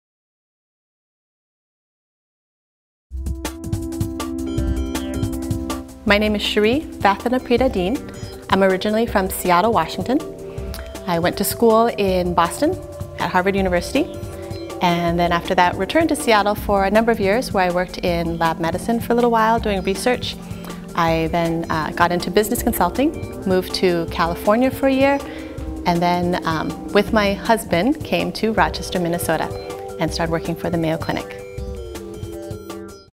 Local Diary Charie Vathanaprida Diehn
Local Diary Charie Vathanaprida Diehn from Seattle talks about her experience in Rochester MN.

Produced by Ben Wehlage

Concept behind spot was to introduce local residences to new faces entering the community.